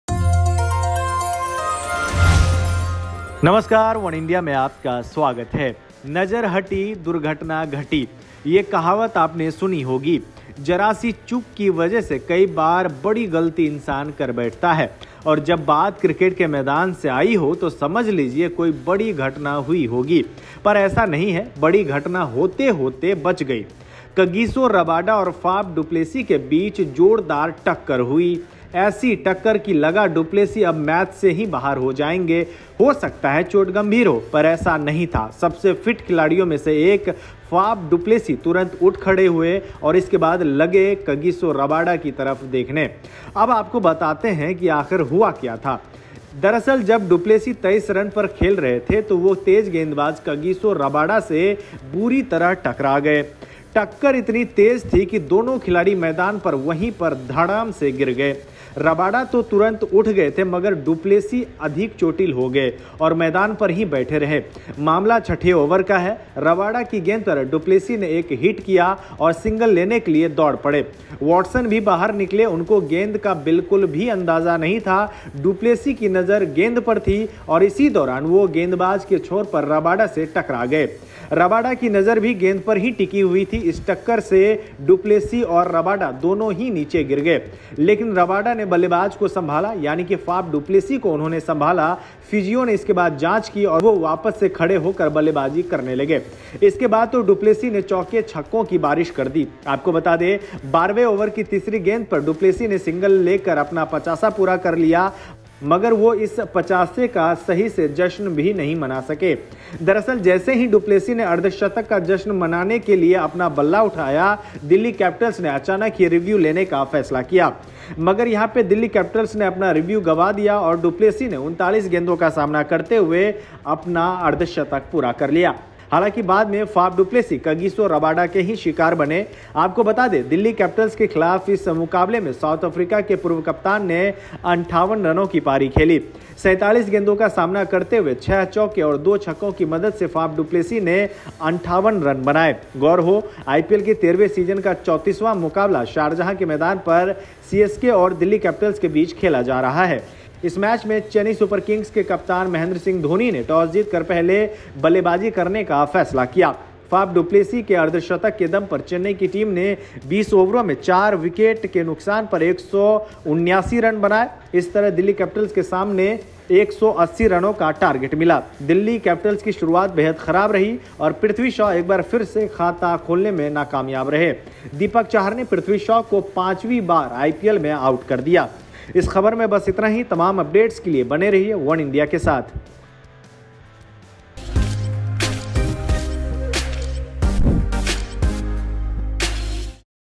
DC vs CSK, IPL 2020 : Faf Du Plessis और Kagiso Rabada में हुई जोरदार टक्कर | वनइंडिया हिंदी
After hitting the ball to mid-on, Faf du Plessis had his eye set on the ball and didn’t see Kagiso Rabada going back to his mark at the bowler’s end, leading to his crash with the fast bowler. Rabada, after making sure that du Plessis was okay, laughed at the incident along with the CSK batsman. On the penultimate delivery of the 5th over, Faf managed to push Kagiso's slower delivery and rushed to take a single. However, the du Plessis and Rabada ended up colliding with each other after both the players were left ball watching and not concentrating on what is in front of them.

नजर हटी दुर्घटना घटी. ये कहावत तो आपने सुनी ही होगी. ज़रा सी चूक की वजह से कई बार बड़ी गलती इन्सान कर बैठता है. और जब बात क्रिकेट के मैदान से आई हो तो समझ लीजिये, कोई बड़ी घटना ही हुई होगी. पर ऐसा नहीं है. बड़ी घटना होते-होते बच गयी. कगिसो रबाडा और फाफ डू प्लेसी के बीच जोरदार टक्कर. ऐसा टक्कर की लगा डू प्लेसी अब मैच से ही बाहर हो जाएंगे. हो सकता है चोट गंभीर हो. पर ऐसा नहीं था. सबसे फिट खिलाडियों में से एक फाफ डू प्लेसी तुरंत उठ खड़े हुए. और इसके बाद लगे रबाडा की तरफ देखने. अब आपको बताते हैं कि आखिर हुआ क्या था? जब प्‍लेसी 23 रन पर खेल रहे थे तो वह गेंदबाज कगिसो रबाडा से बुरी तरह टकरा गए.